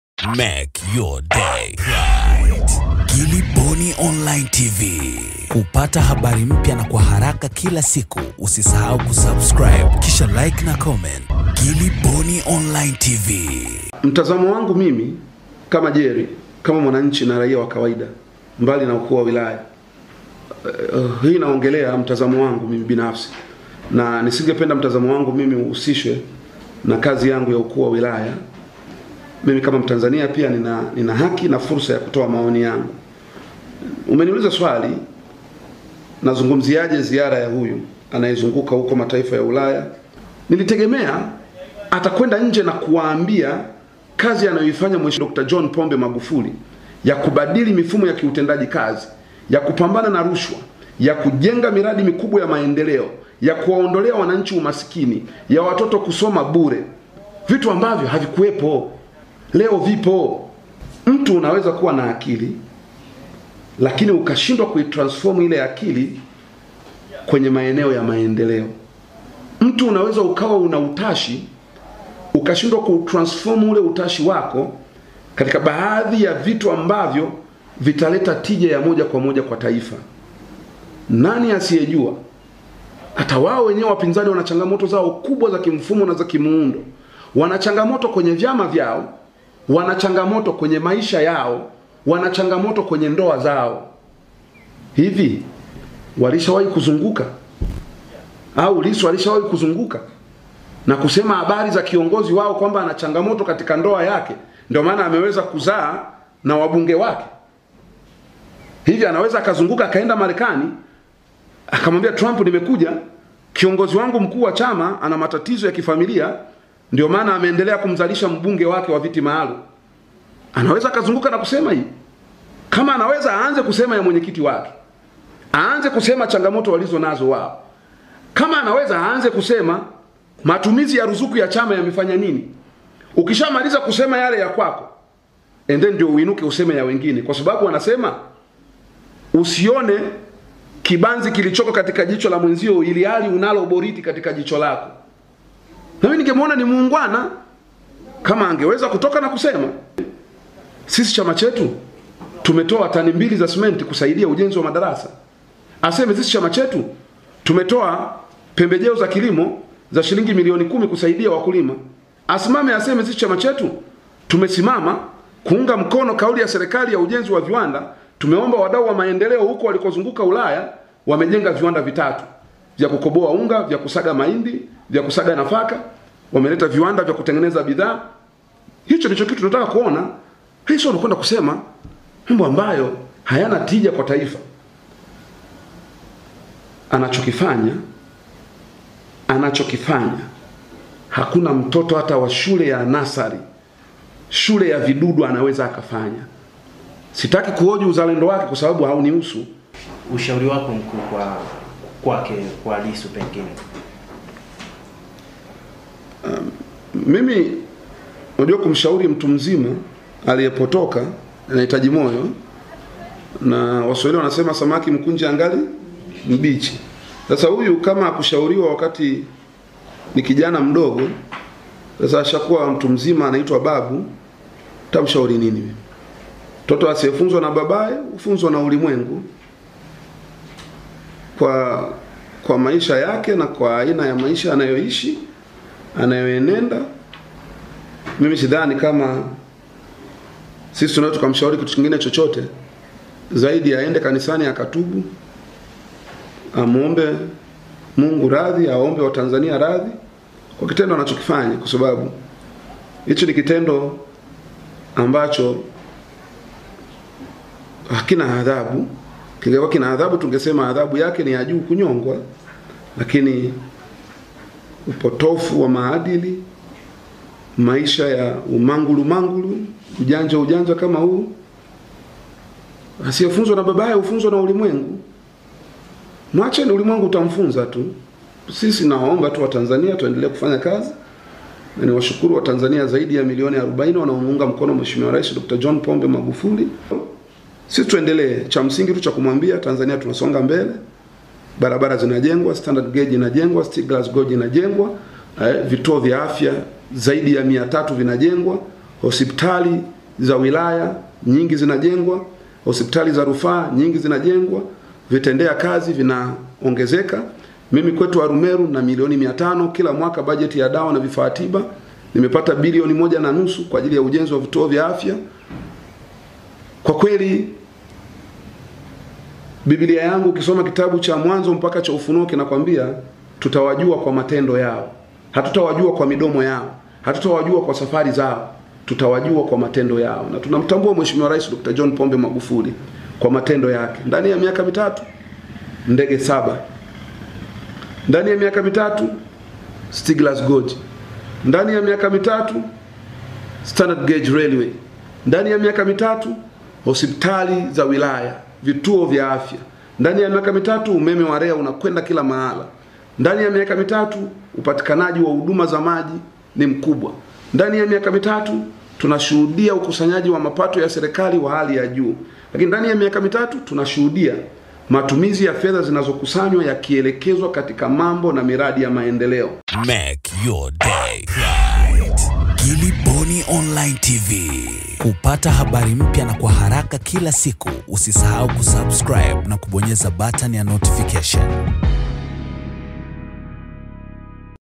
Ghafla! Tundu Lissu Amepata Kiboko yake, Jerry Muro Amuumbua Vibaya, CHADEMA nayo Haijabaki Salama
Mkuu wa wilaya ya Arumeru Jerry Muro amezungumza na Gilly Bonny Online Tv kuhusu ziara anazofanya mbunge wa Singida Mashariki - CHADEMA Tundu Lissu Barani Ulaya.